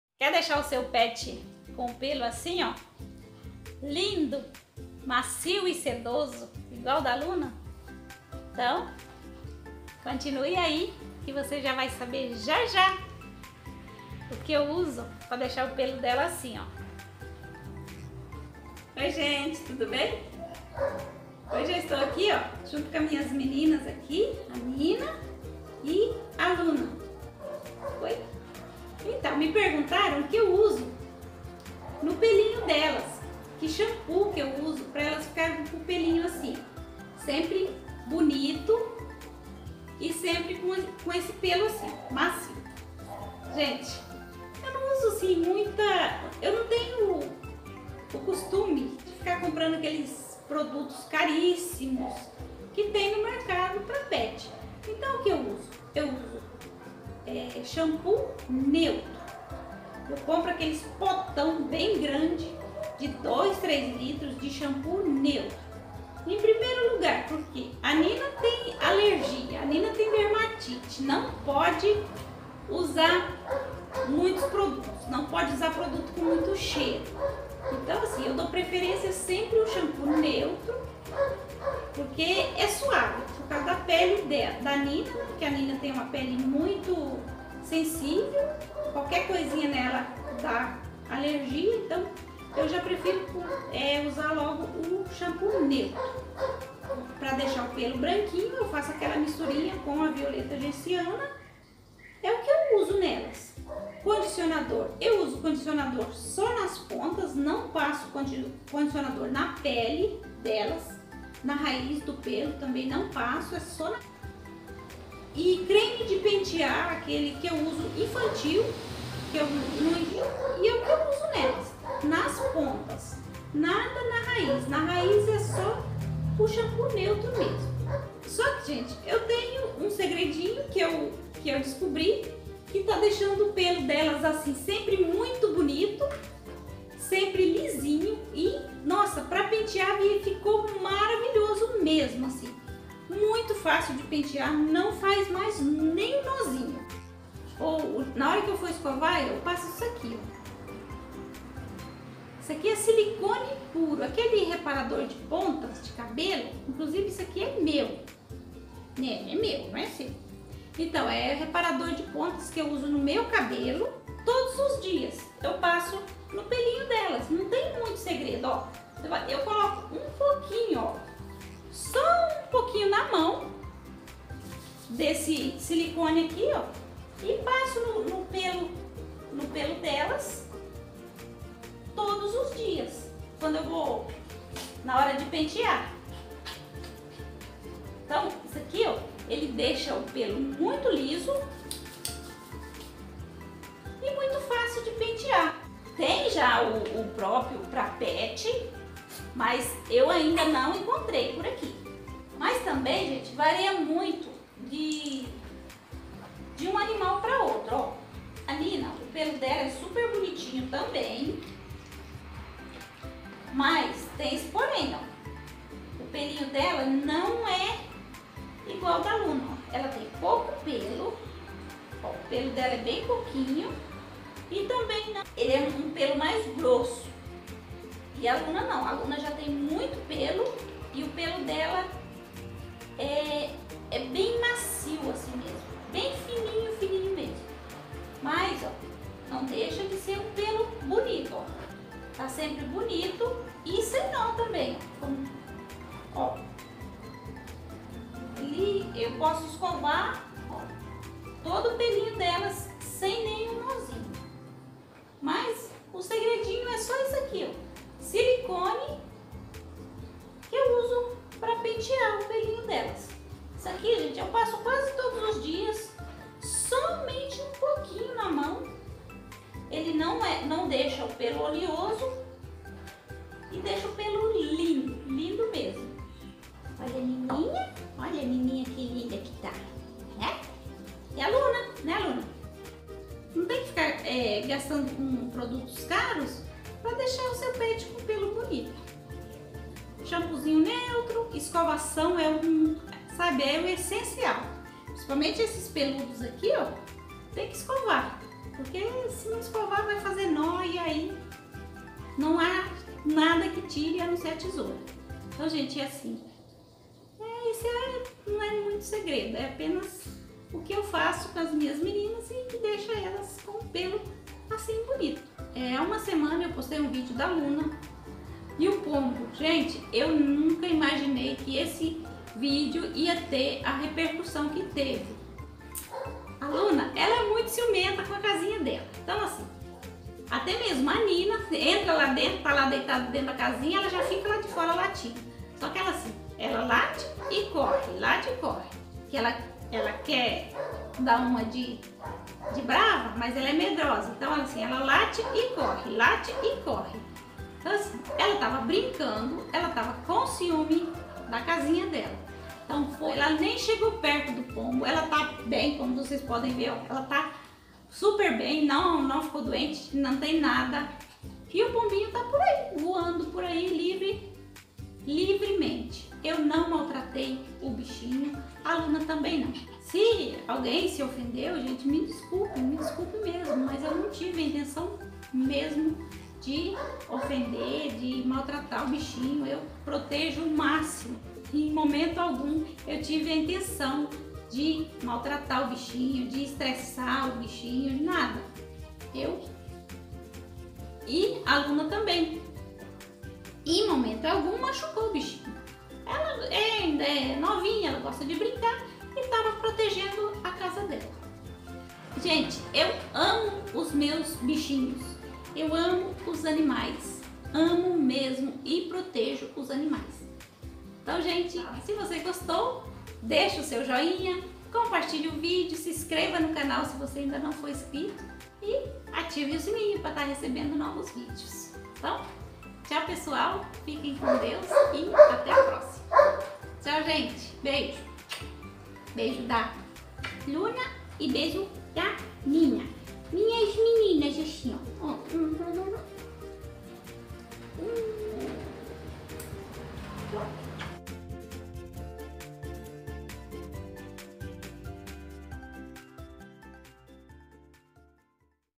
Pelos lindos macios brilhantes e sem nós ( Sem produtos caros)
Gostaria de deixar seu pet com os pelos macios,  brilhante e sem Nós? Então continue assistindo o vídeo que tenho certeza de que você vai gostar da dica de hoje . Como deixar o pelo do seu pet lindo  sem gastar com produtos caros.